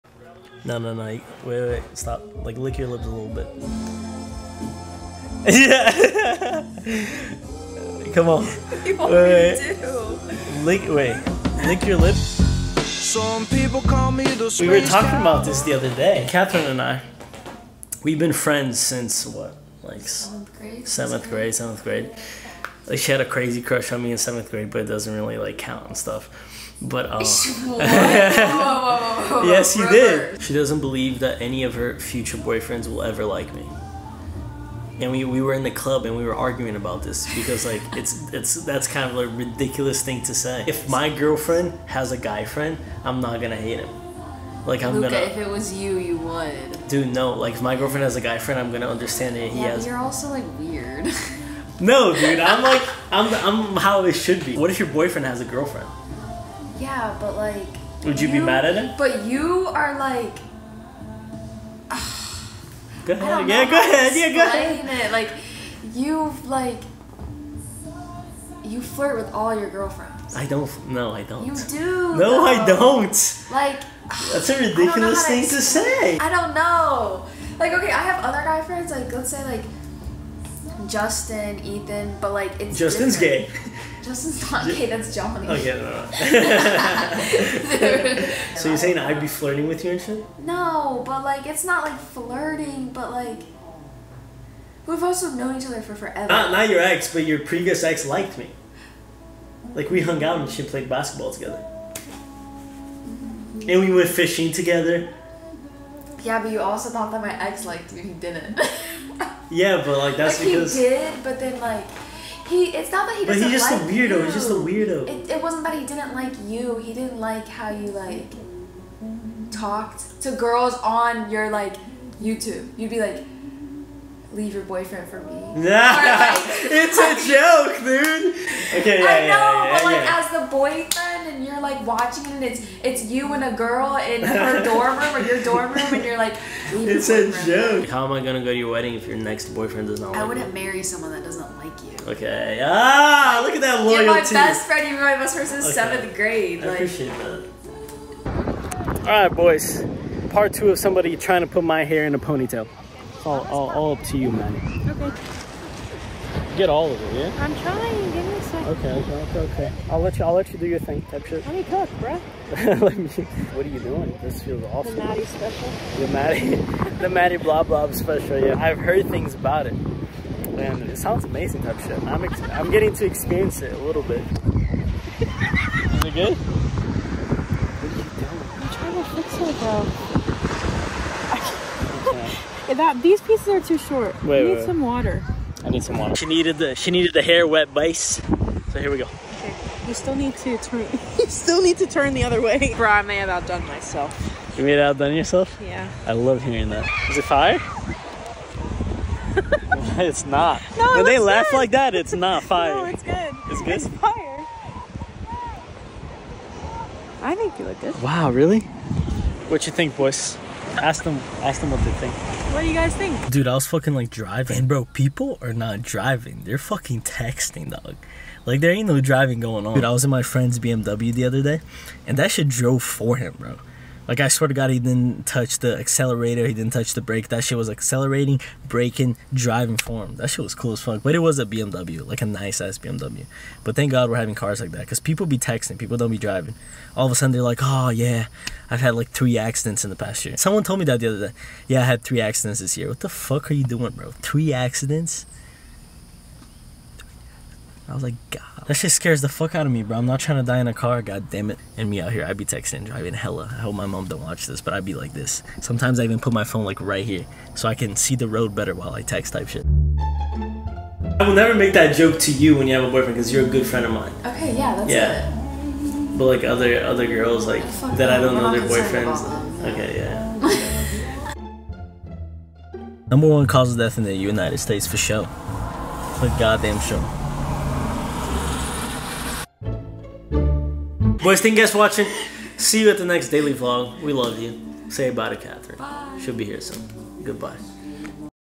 "Does Your BoyFriend Like Me?"
Welcome to vlog 250!!! In this episode we talk about our future relationships, texting while driving, and pony tails.

Timecodes:
00:00 - Intro
00:14 - BoyFriend
02:28 - Hair Up
03:33 - Texting And Driving
04:19 - Outro